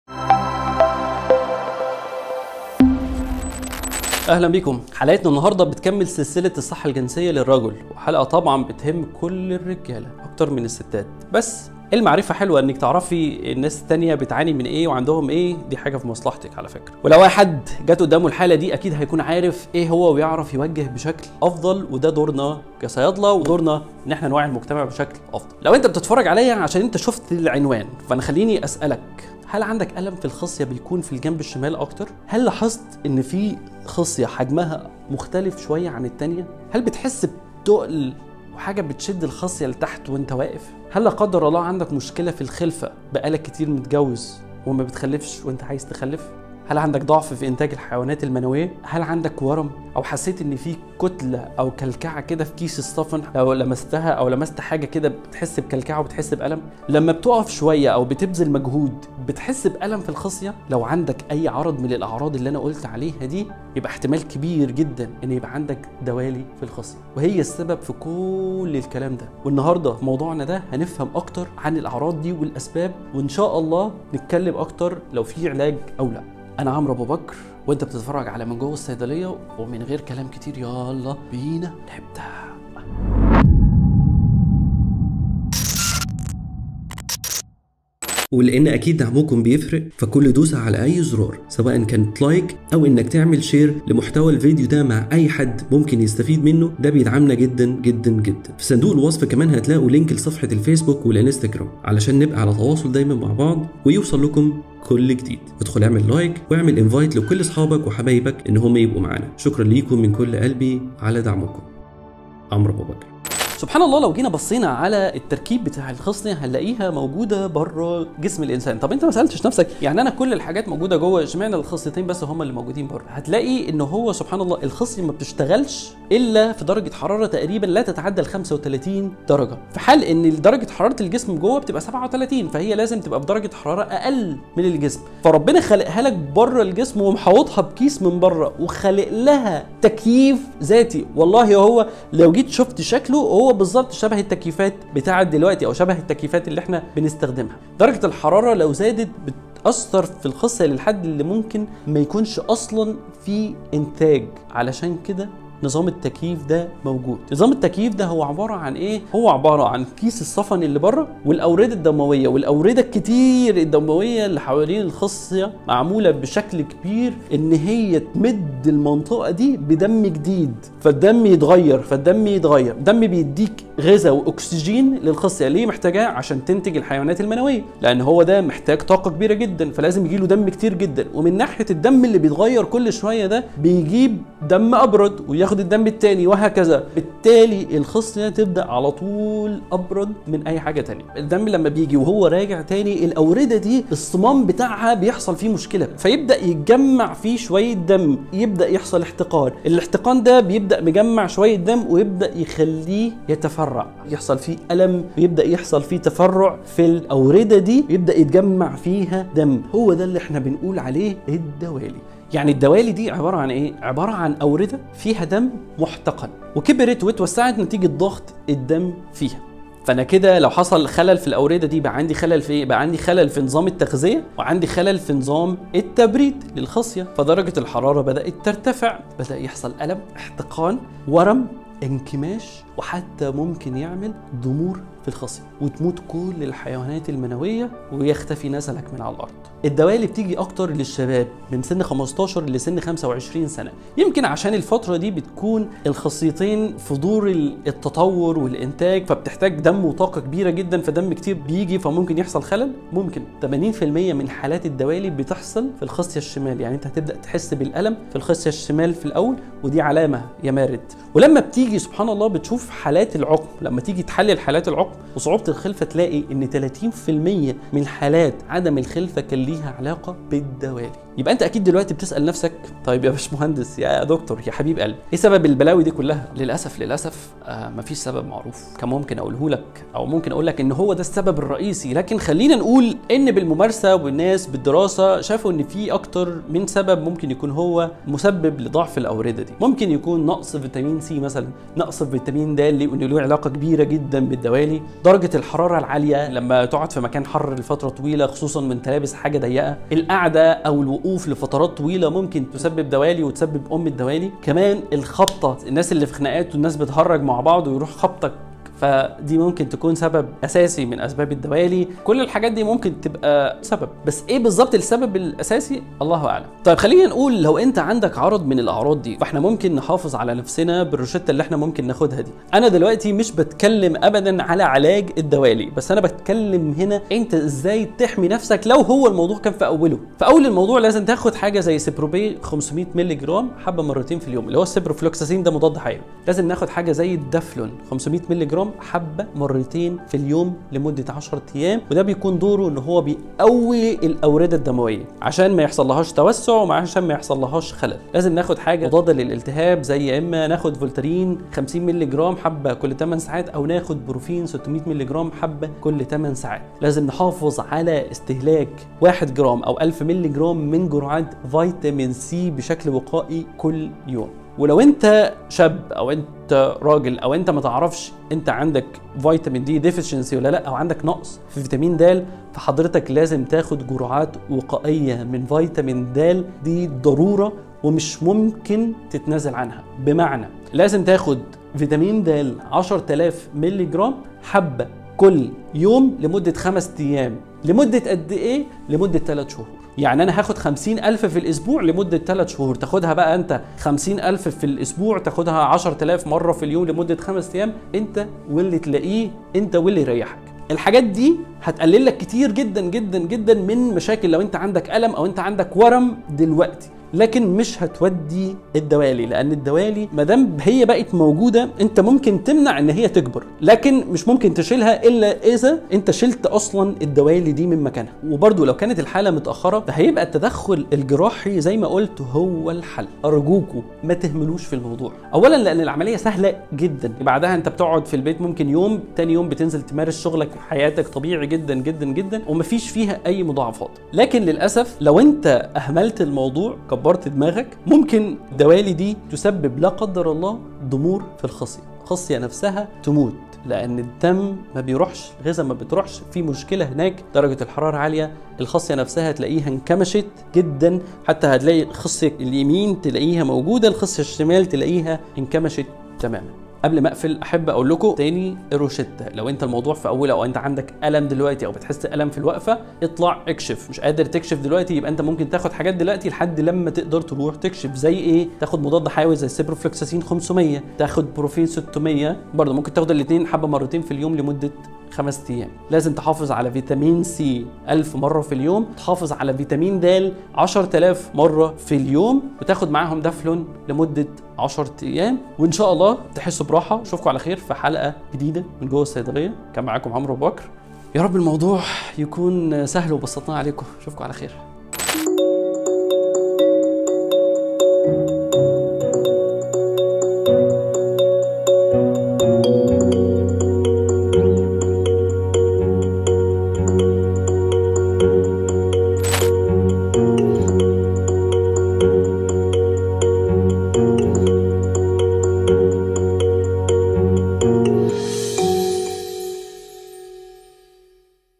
دوالي الخصية | السبب الرئيسي لتأخر الإنجاب
ربنا خلق للخصيه نظام تكيف (مبرد) وهو عباره عن كيس الصفن واورده دمويه كتير جدا وظيفتها على طول تمد المنطقه دى بدم جديد , من ناحيه يدى الغذاء والاكسجين للخصيه اللى محتاجاه علشان تنتج الحيوانات المنويه ومن ناحيه تانيه نظام تبريد
الدم ده وهو راجع من الخصيه بيحصل مشكله فى صمام الاورده , فالدم بيتجمع ويحصل احتقان والاورده تكبر وتتوسع , مع الوقت بيحصل خلل فى اورده كثيره وبكده بيحصل خلل فى نظام التغذيه ونظام التبريد للخصيه , درجه الحراره بترتفع فبيحصل الم , احتقان , ورم , انكماش وضمور فى الخصيه , وتموت الحيوانات المنويه
وبتيجى اكثر للشباب من سن 15 سنه – 25 سنه , يمكن علشان الفتره دى بتكون الخصيتين فى دور التطور والانتاج فبيحتاج دم وطاقه أكثر..
80 % من حالات الدوالى بتحصل فى الخصيه الشمال بس , ولما بتيجى تشوف حالات العقم وصعوبه الخلفه , تلاقى ان 30 % منها
ليها علاقه بالدوالى

0:00 نظرة عامة على الموضوع
1:54 فسيولوجيا ووظيفة الخصيتين
3:10 دوالي الخصية
3:48 أعراض دوالي الخصية
3:59 أين هي أكثر شيوعًا؟
4:33 ما الذي يسبب المشكلة؟
5:17 الوقاية والحماية من دوالي الخصية
7:36 ملخص

متنسااش تتفرج علي الحلقات دي